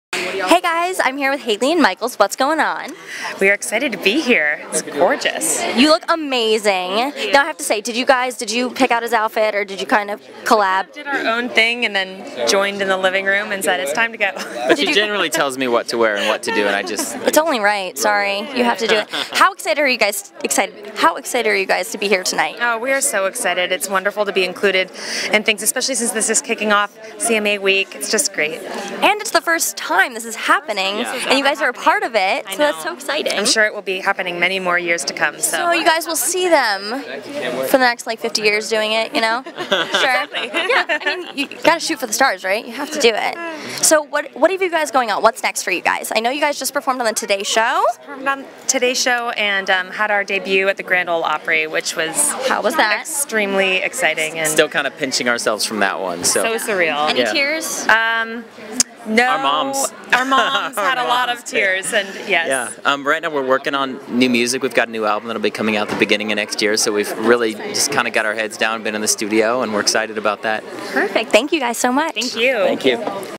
Haley and Michaels Interview Off The Record Nashville Red Carpet
We got to interview Haley & Michaels in Nashville at Off The Record's red carpet.

We’re Stage Right Secrets we interview your favorite artists, bands, and entertainers with your questions.